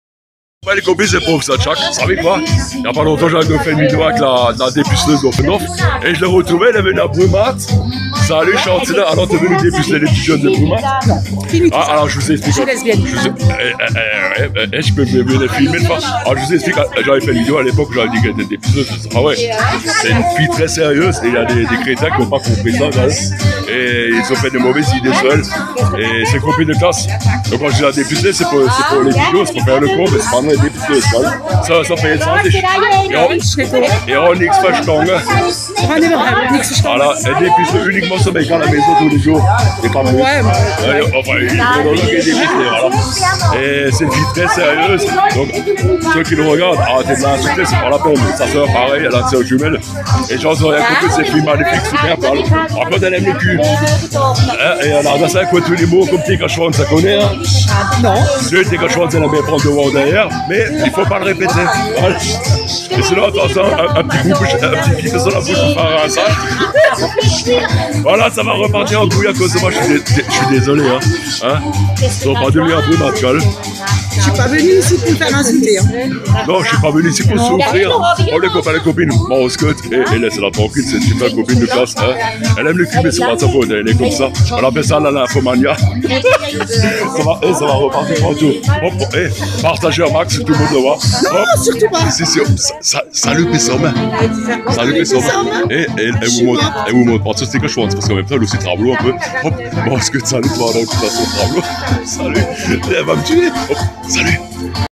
La nymphomane dépuceleuse de retour avec le Professeur Djack d' Alsace
Mais quelle bonne surprise de retrouver une de mes fans de Offendorf et copine de classe qui a est une fois apparu dans une mes vidéos sous le titre sulfureux de dépuceleuse du village.
Ce titre lui a valu quelques critiques déplacés par ceux qui ne comprenaient pas l'humour du sketch.
On en remet une couche pour bien expliquer a ceux qui n'auraient pas compris qu'elle est cochonne que chez elle mdr (je pouvais pas m'en empêcher).
Partagez un max cette vidéo, c'est Manu qui vous le demande.